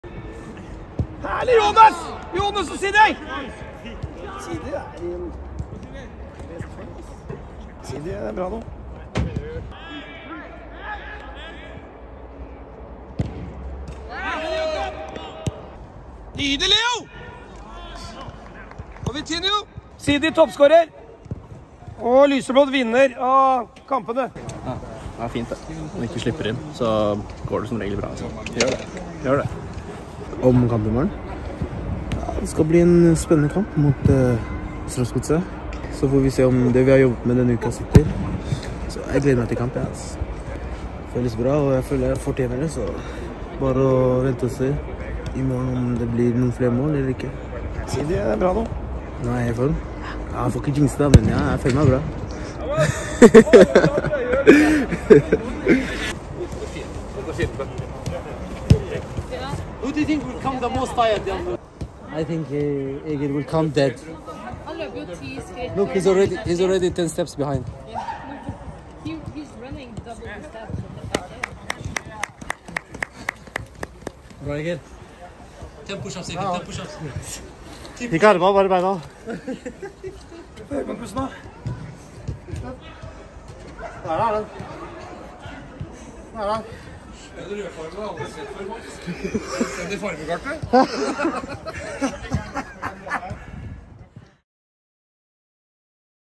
- Jeg føler jeg fortjener det
Seedy Jatta starter på topp når Strømsgodset gjester Intility Arena fredag formiddag.